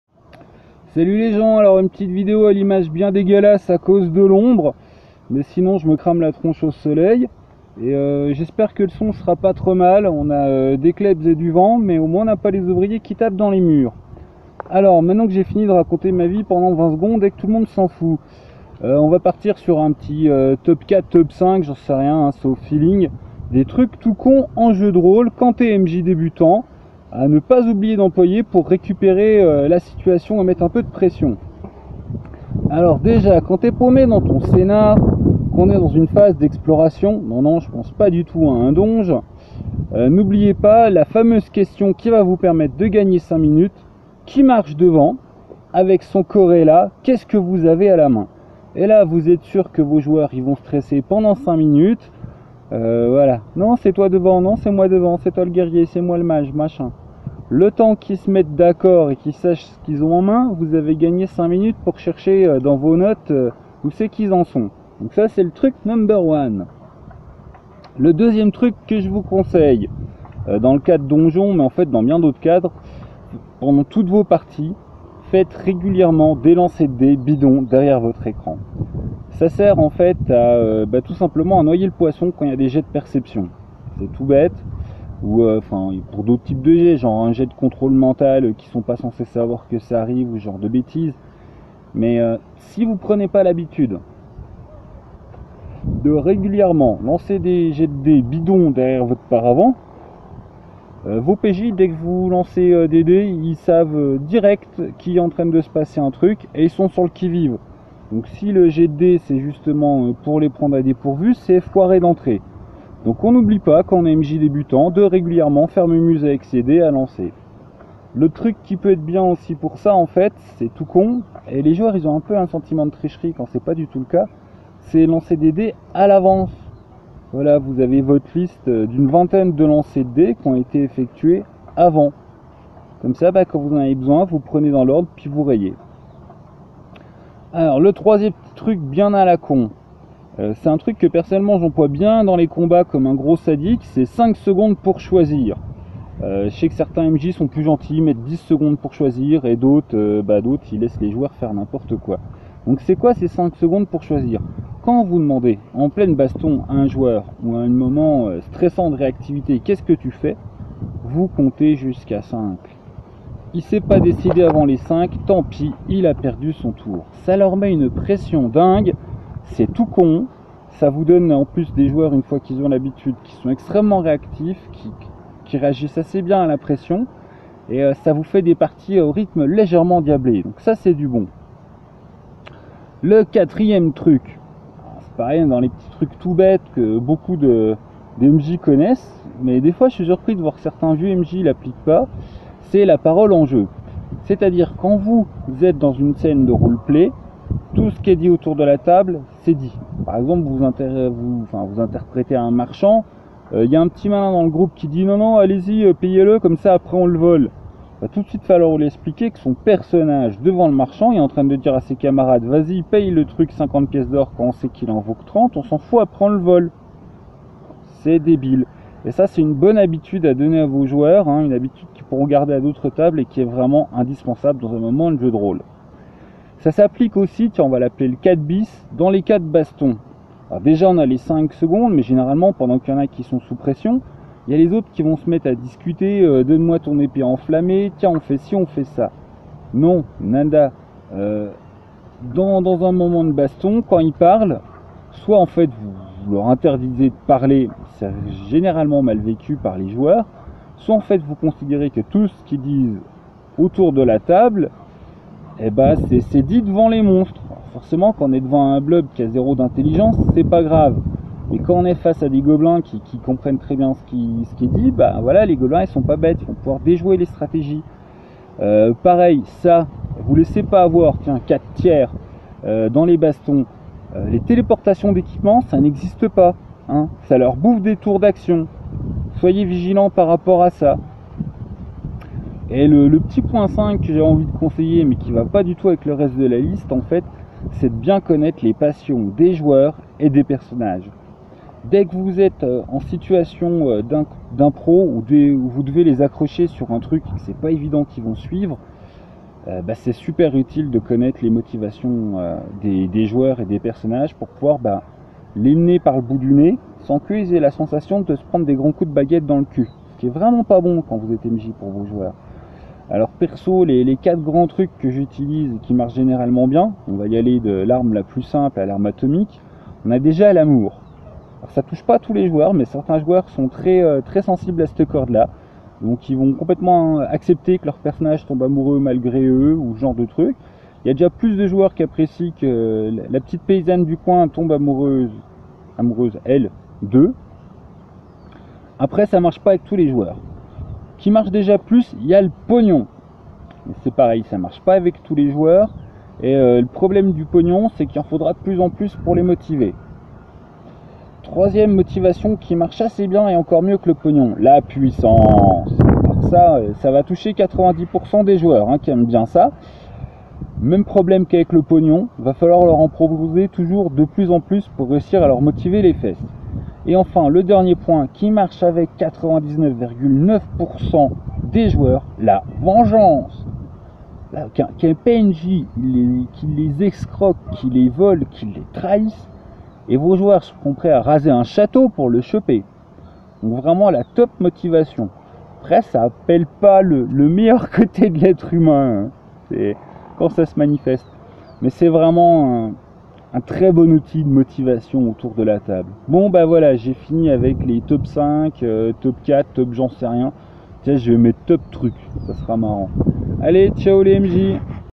TOP Trucs pour MJ débutant
Un top avec des trucs et astuces pour meujeu néophyte... avec du vent en arrière fond aussi parfois et ça c'est dégueulis.